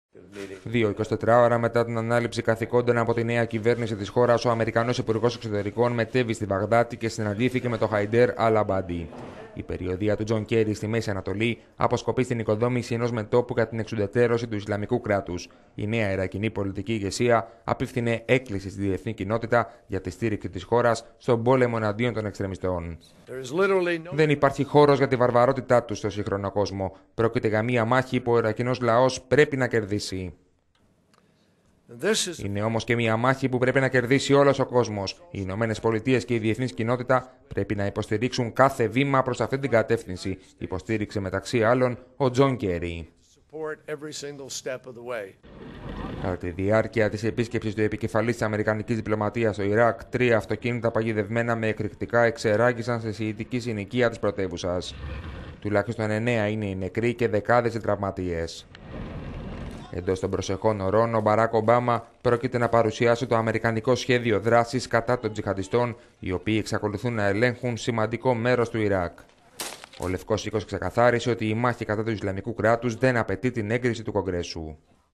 Αντίστροφη μέτρηση για το σχέδιο Ομπάμα κατά των τζιχαντιστών
Δυο ημέρες μετά την ανάληψη καθηκόντων από τη νέα κυβέρνηση του Ιράκ, ο Αμερικανός υπουργός Εξωτερικών μετέβη στη Βαγδάτη και συναντήθηκε με τον Χαϊντέρ Αλ Αμπάντι.

Η περιοδεία του Τζον Κέρι στη Μέση Ανατολή αποσκοπεί στην οικοδόμηση ενός μετώπου για την εξουδετέρωση του Ισλαμικού Κράτους.

Η νέα ιρακινή πολιτική ηγεσία απηύθυνε έκκληση στη διεθνή κοινότητα για την στήριξη της χώρας στον πόλεμο εναντίον των εξτρεμιστών.

«Δεν υπάρχει χώρος για την βαρβαρότητά τους στο σύγχρονο κόσμο. Πρόκε…